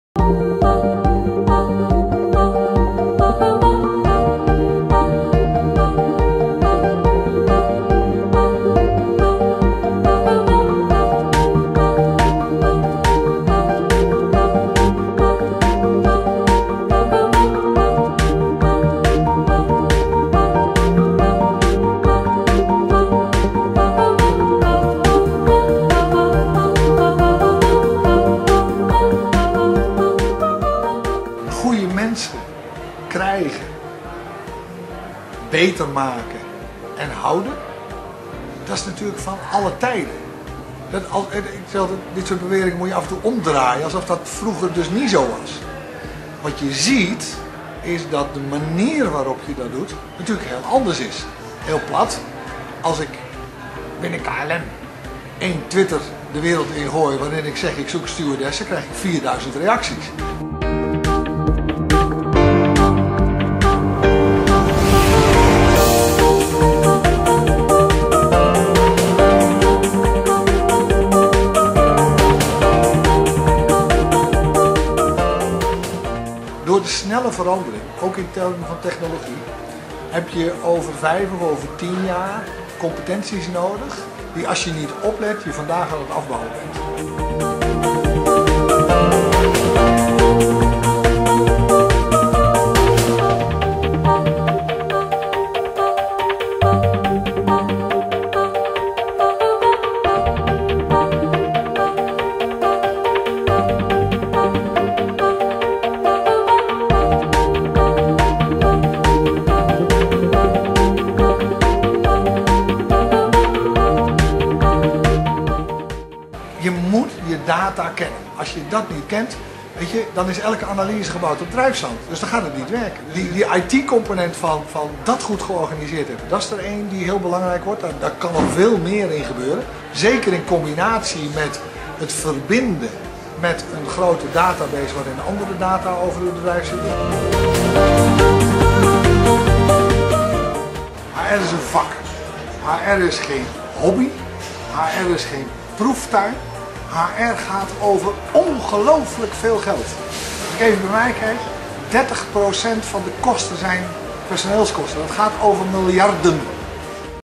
RAET HR trends, HR Summit
Op 7 en 8 februari vond de tweede editie van de Strategie Summit voor HR-directeuren plaats op landgoed Duin & Kruidberg. De Strategie Summit bevordert de dialoog tussen HR-eindverantwoordelijken uit zowel de publieke sectoren als het bedrijfsleven en biedt hen een platform om te netwerken, kennis te delen en kennis te vergaren.